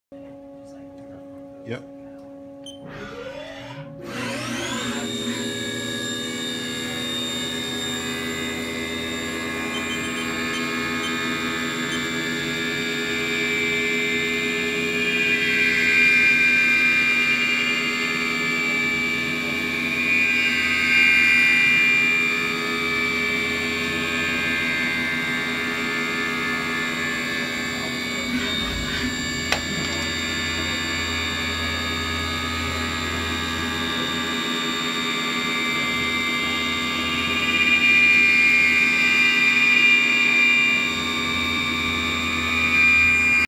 CNC Finishing Cut
CNC Machining of upper plate of custom Triple Tree for BarelyABike.com.  Thanks to Lars at ODU Machine Shop